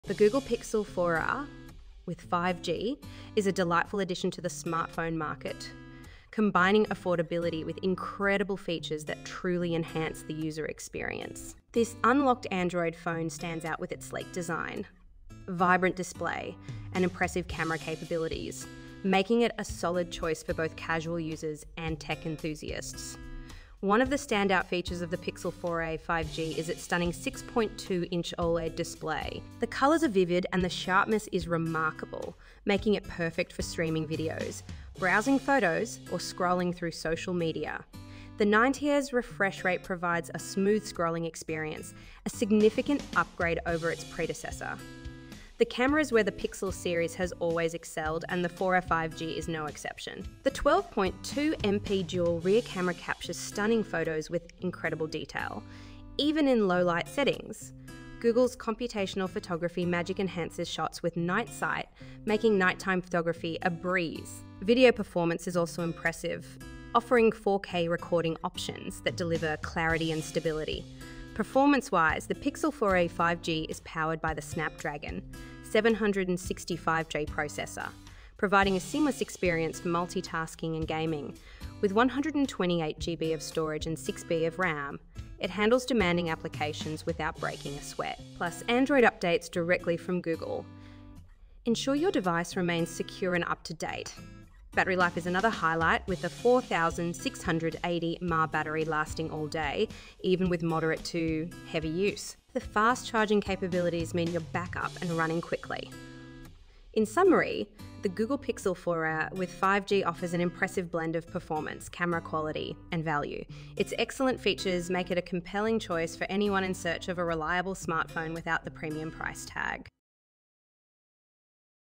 Google Pixel 4a with 5G Android Phone New Unlocked Smartphone Review
The Google Pixel 4a with 5G is a delightful addition to the smartphone market, combining affordability with incredible features that truly enhance the user experience. This unlocked Android phone stands out with its sleek design, vibrant display, and impressive camera capabilities, making it a solid choice for both casual users and tech enthusiasts.

One of the standout features of the Pixel 4a 5G is its stunning 6.2-inch OLED display. The colors are vivid, and the sharpness is remarkable, making it perfect for streaming videos, browsing photos, or scrolling through social media. The 90Hz refresh rate provides a smooth scrolling experience, a significant upgrade over its predecessor.

The camera is where the Pixel series has always excelled, and the 4a 5G is no exception. The 12.2 MP dual rear camera captures stunning photos with incredible detail, even in low-light settings. Google's computational photography magic enhances shots with Night Sight, making nighttime photography a breeze. Video performance is also impressive, offering 4K recording options that deliver clarity and stability.

Performance-wise, the Pixel 4a 5G is powered by the Snapdragon 765G processor, providing a seamless experience for multitasking and gaming. With 128GB of storage and 6GB of RAM, it handles demanding applications without breaking a sweat. Plus, Android updates directly from Google ensure your device remains secure and up-to-date.

Battery life is another highlight, with the 4680 mAh battery lasting all day, even with moderate to heavy use. The fast-charging capabilities mean you're back up and running quickly.

In summary, the Google Pixel 4a with 5G offers an impressive blend of performance, camera quality, and value. Its excellent features make it a compelling choice for anyone in search of a reliable smartphone without the premium price tag.